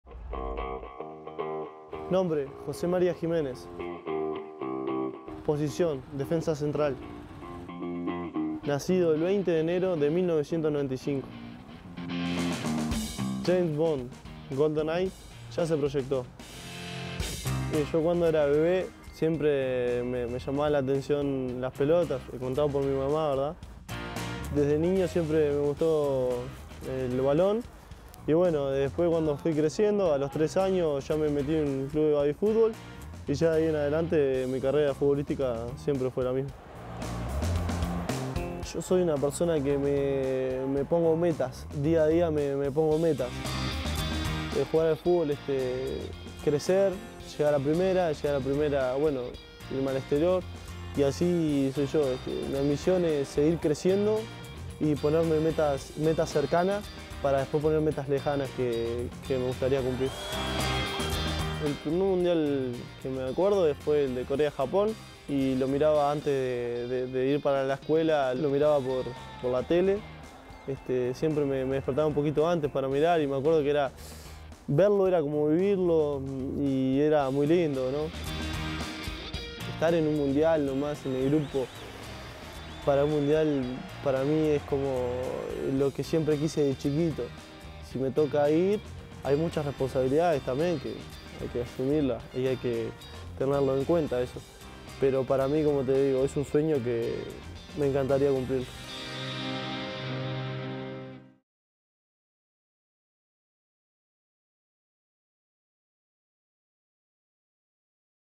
Brazil 2014 Stars to Watch: Jose Maria Gimenez
A driven central defender, Jose Maria Gimenez is hoping to build on success at the U-20 FIFA World Cup in 2013 by emerging at the 2014 FIFA World Cup Brazil™.